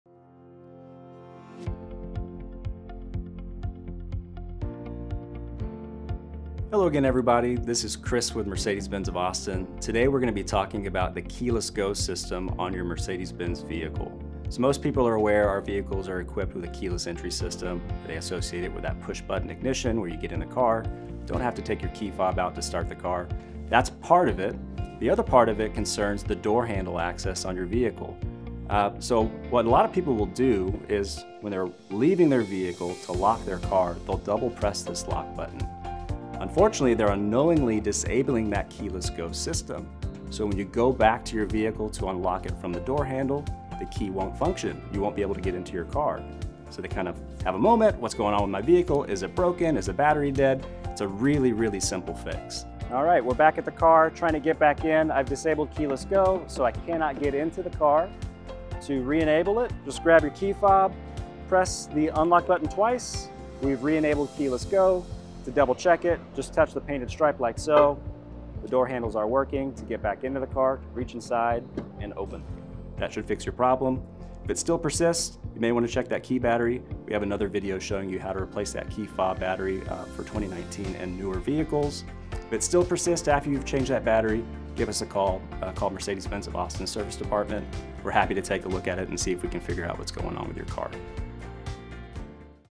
KEYLESS GO How it Works Mercedes-Benz
Mercedes-Benz of Austin's Chris Hernandez explains the many uses of Keyless Go.

Mercedes-Benz of Austin - Always Moving Forward.